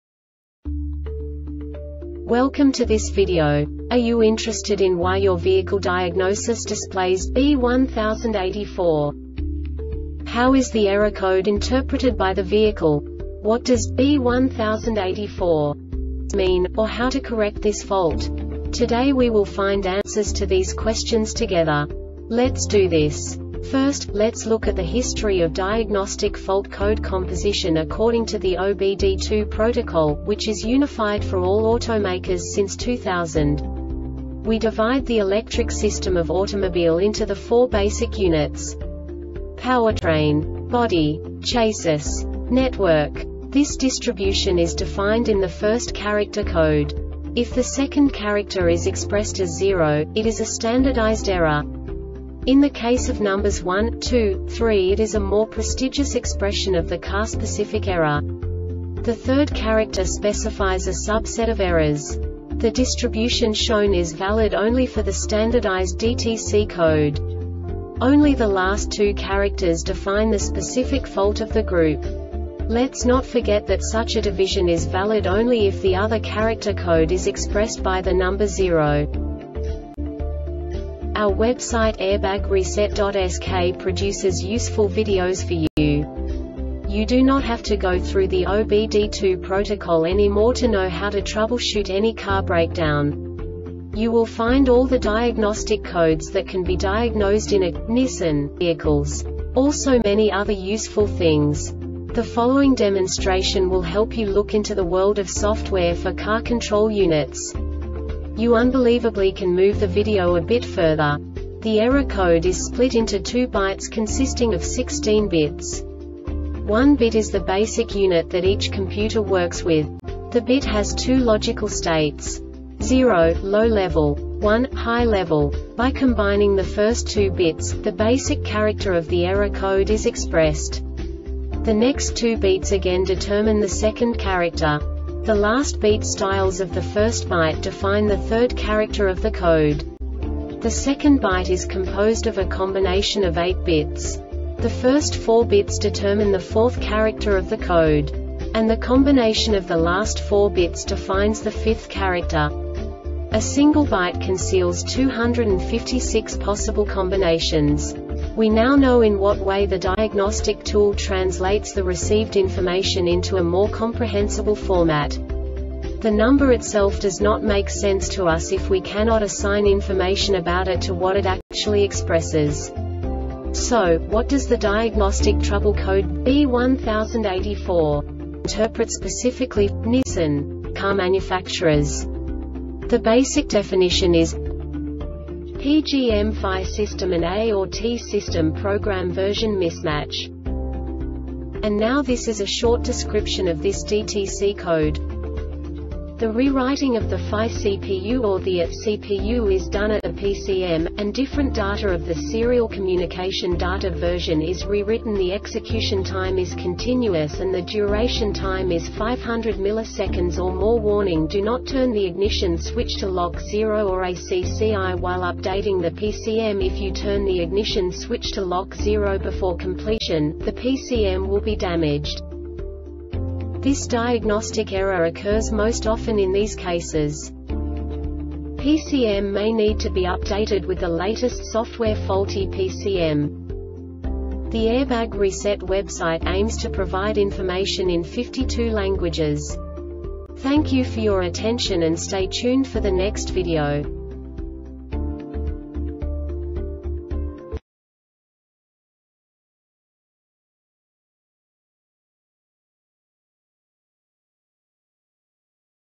DTC Nissan B1084 Short Explanation
Contents:
0:21 Basic DTC analysis according to OBD2 protocol standard.
1:48 Insight into programming
2:58 A diagnostically understandable description of the Diagnostic Trouble Code
3:05 Basic error definition

Make:
Nissan
Code:
B1084
Definition: